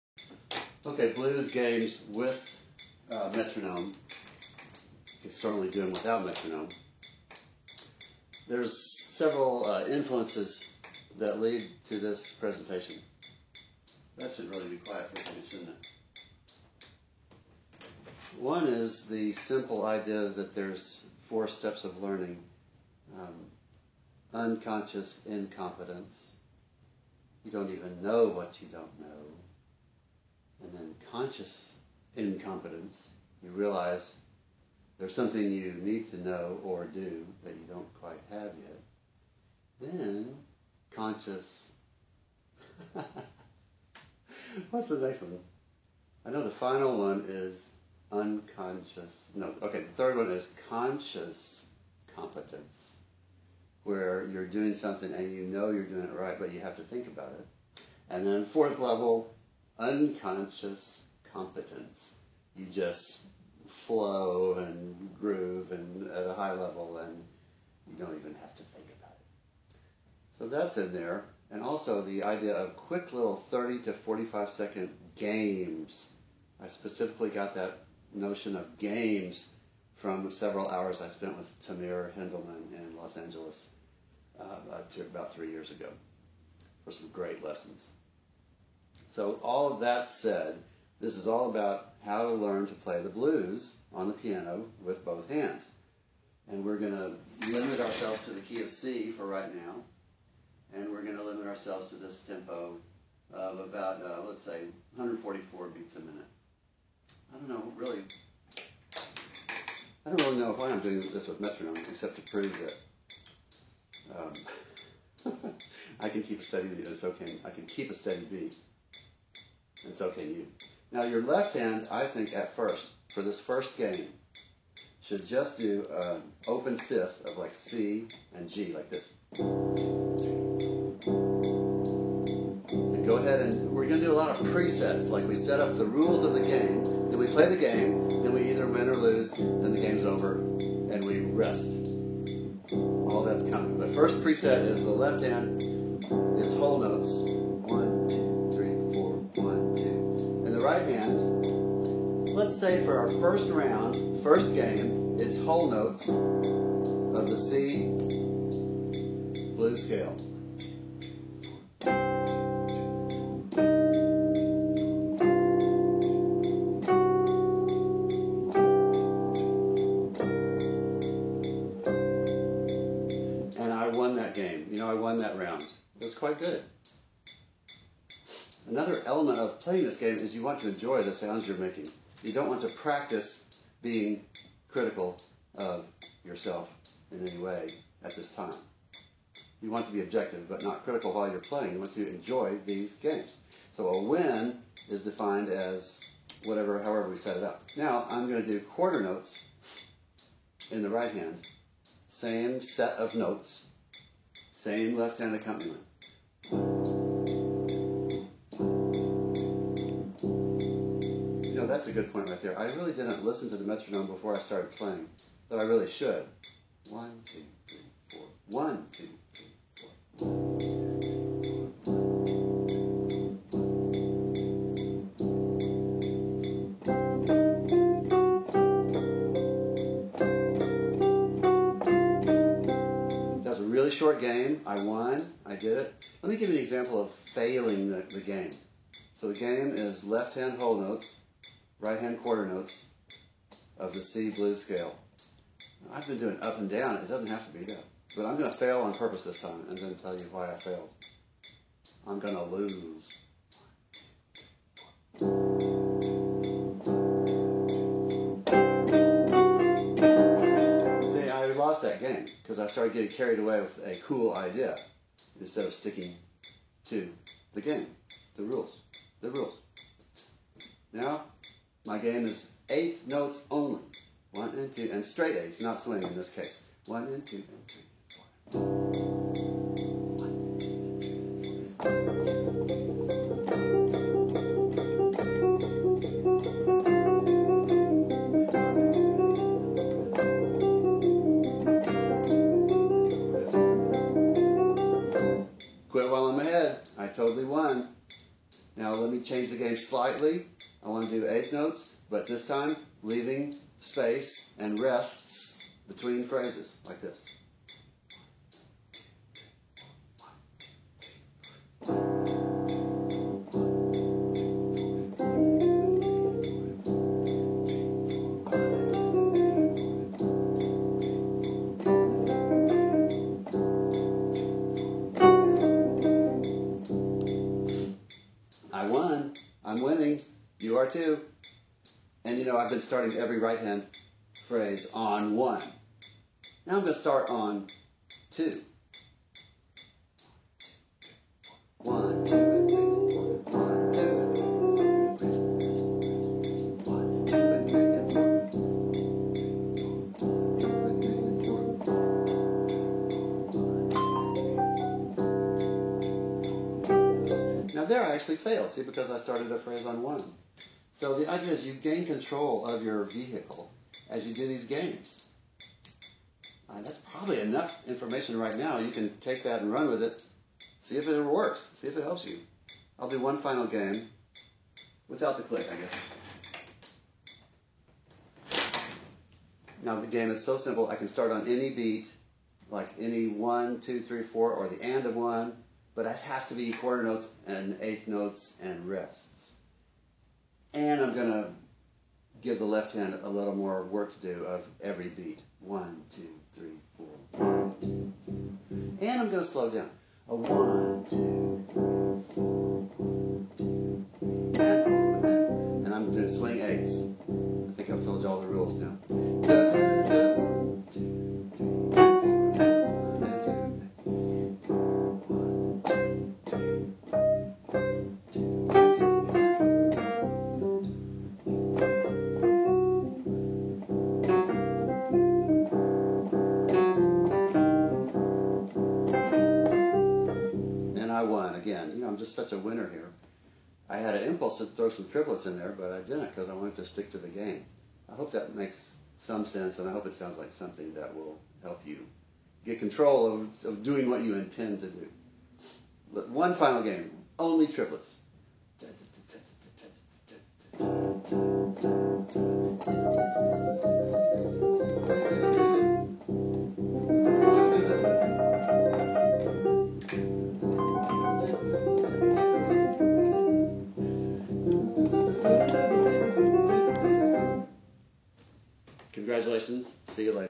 Piano Blues Learning Games
Beginner Level Piano Blues, key of C, basic method of getting started and a great way to develop blues skills. Prerequisites: You need some skill in knowing the notes of the blues scale, and some skill in creating the rules for each game. Once you have that, you can really exercise and develop your chops with games like this. There are fifty thousand things this video does NOT cover, like: the blues progression, Left Hand variants, etc, etc...This video focuses on how to play your intended RH phrases along with LH, get away from cliches, and how to develop your improv skills with short games.

Let me know if it helped, or exactly what would help more!